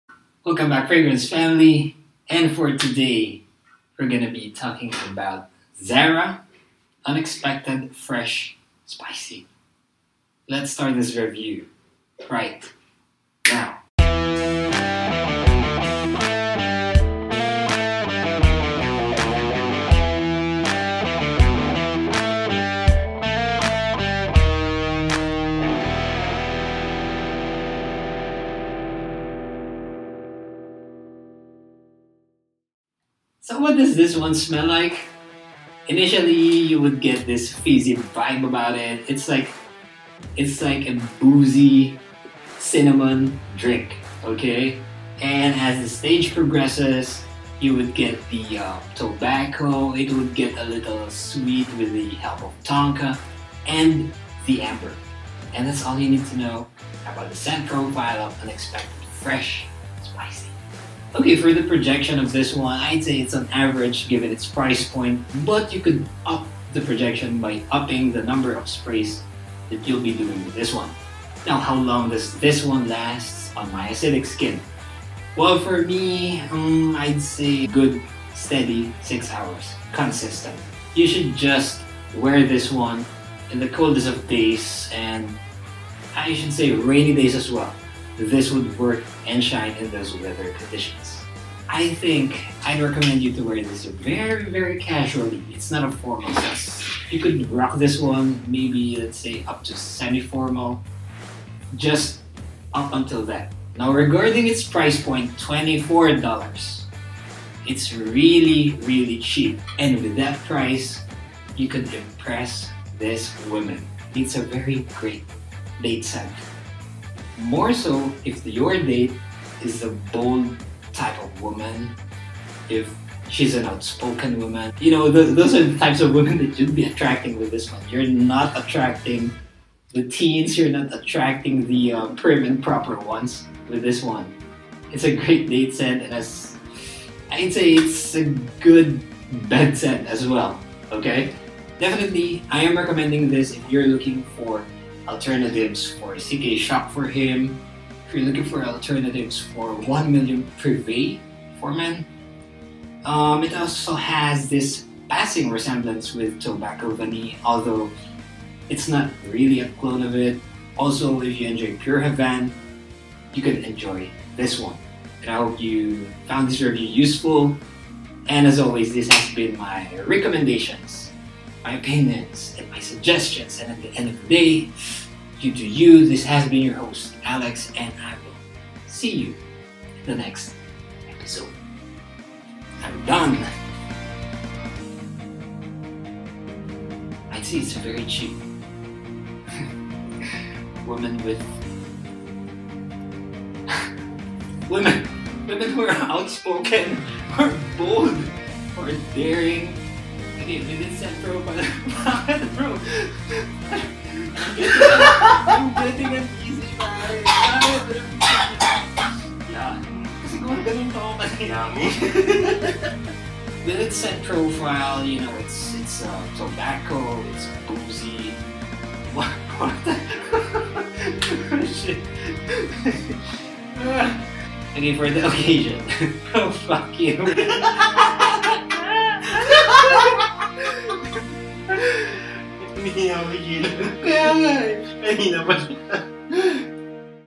Unexpected Fresh Spicy by Zara review
Law of Economics - Supplies and prices of Unexpected Fresh Spicy by Zara don't lie. It's getting harder & harder to source this gem... but before hoarding this cheapie, is this really for you? Find out in this short review!